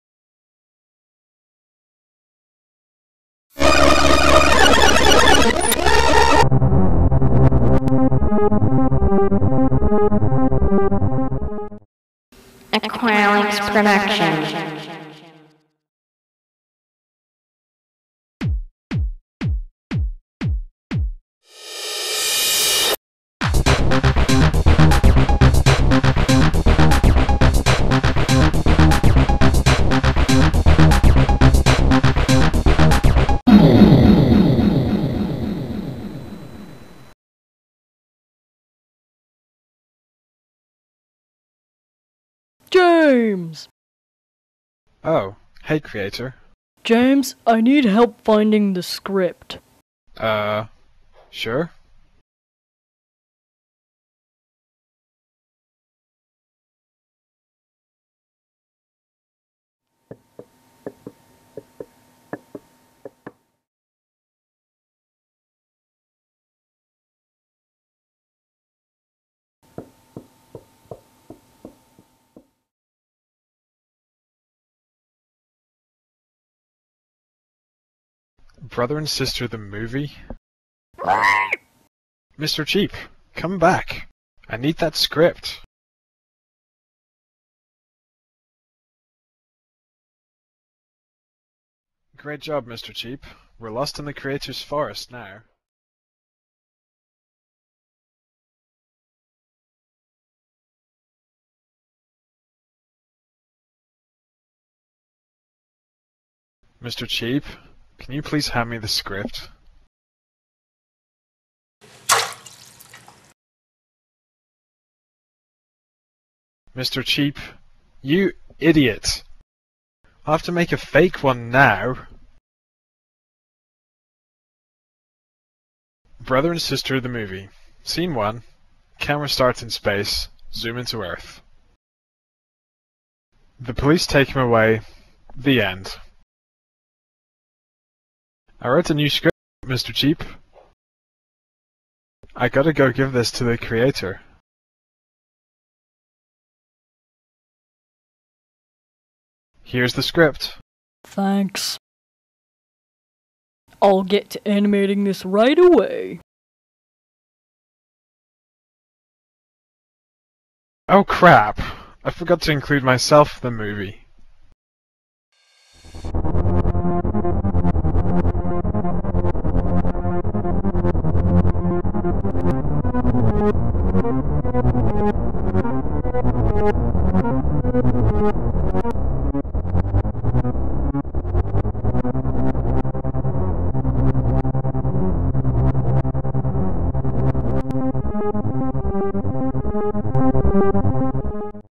Brother and Sister: Episode 9 - Animated Short - Aqlex Productions
Updates to the show: New backgrounds, New Items.
Sorry it took so long for the next episode to come up, there was a bit of a delay. This episode is all about James, so enjoy!